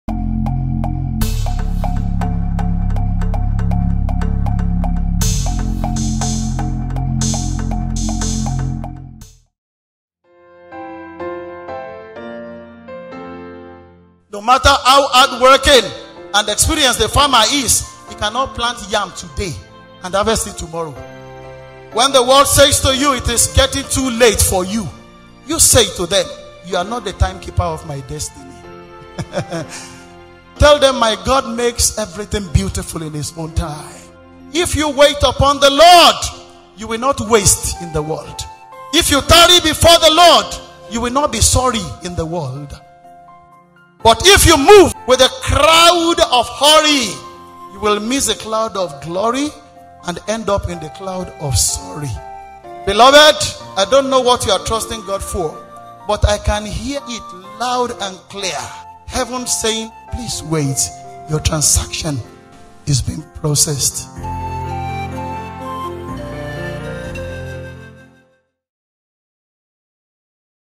SMART (Eps 1) // Wait for the Cloud - By Shola Mike Agboola // Short, sharp & direct message // EVOM
'SMART' series consists of short excerpts from messages preached by Bro. 'Shola Mike Agboola. In this episode, the preacher admonishes the viewer not to be pressured out of God's will and timing.